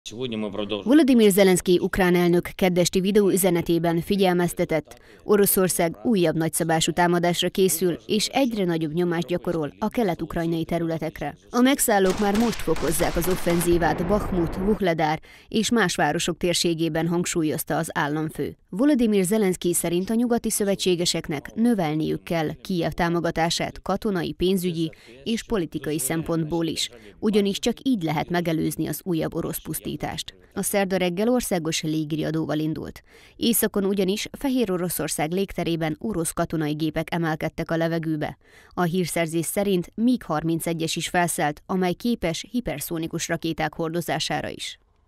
Tv21 Ungvár - Volodimir Zelenszkij: Oroszország újabb nagyszabású támadásra készül
Volodimir Zelenszkij ukrán elnök kedd esti videó üzenetében figyelmeztetett: Oroszország újabb nagyszabású támadásra készül, és egyre nagyobb nyomást gyakorol kelet-ukrajnai területekre. A megszállók már most fokozzák az offenzívát Bahmut, Vuhledar és más városok térségében hangsúlyozta az államfő. Volodimir Zelenszkij szerint a nyugati szövetségeseknek növelniük kell Kijev támogatását katonai, pénzügyi és politikai szempontból is, ugyanis csak így lehet megelőzni az újabb orosz pusztítást. A szerda reggel országos légiriadóval indult. Északon ugyanis, Fehéroroszország légterében orosz katonai gépek emelkedtek a levegőbe. A hírszerzés szerint MiG–31-es is felszállt, amely képes hiperszonikus rakéták hordozására is.